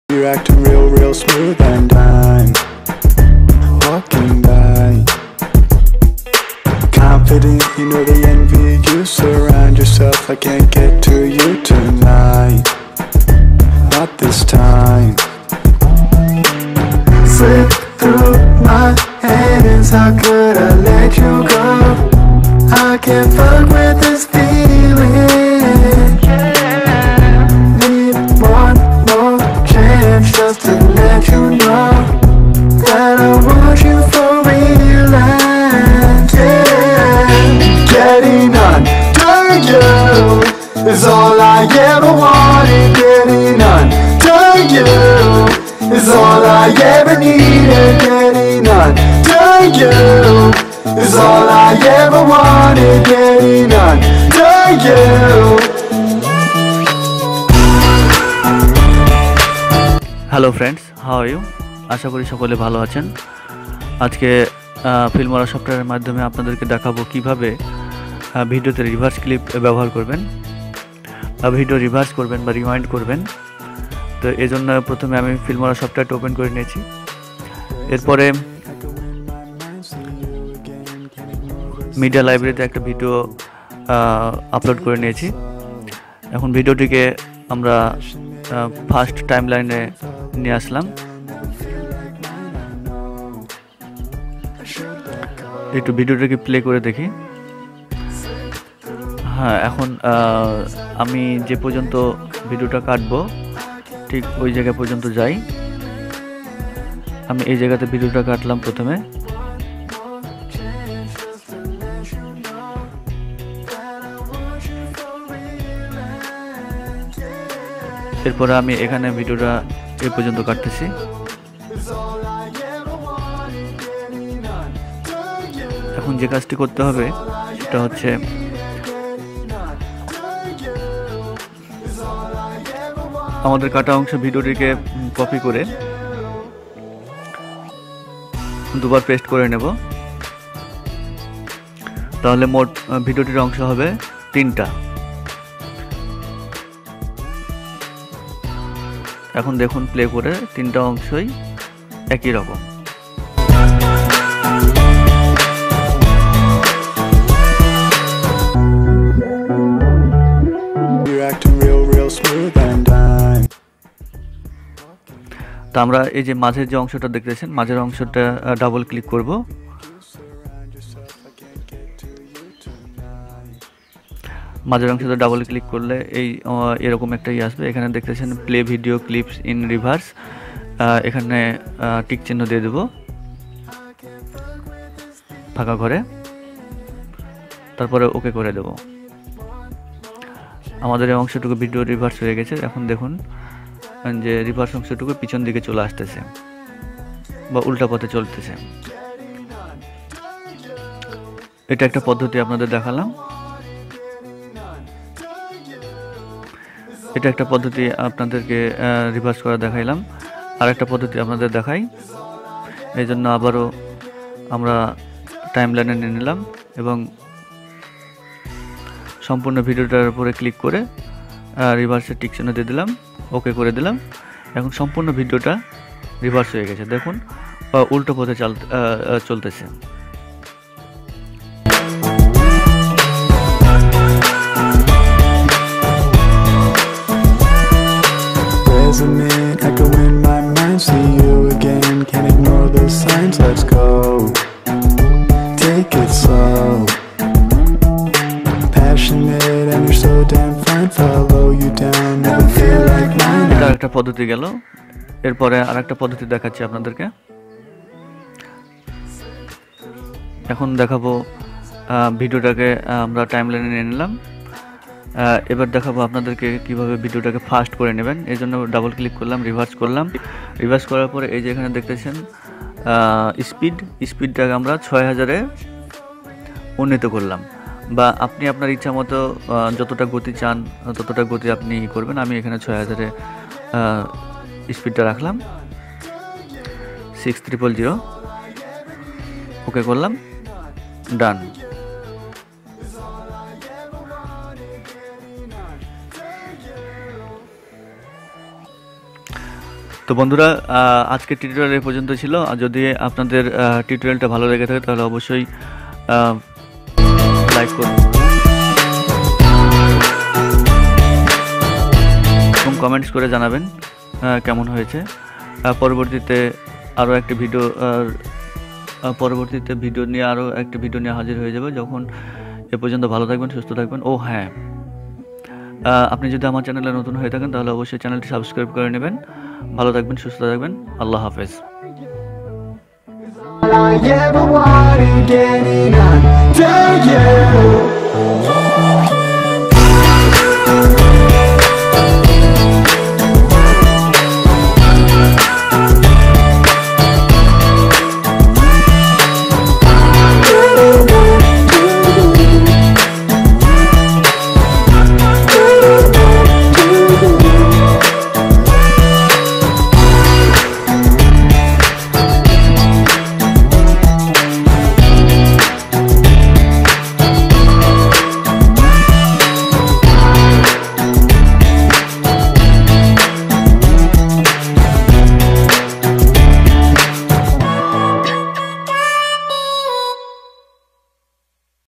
How to make reverse/Rewind video in only one click by Filmora Tutorial -02
How to make reverse/Rewind video in only one click by Filmora Tutorial

Easy create a attractive video by Wondershare filmora.
It is very helpful software.

wondershare filmora filmora filmora tutorial in hindi,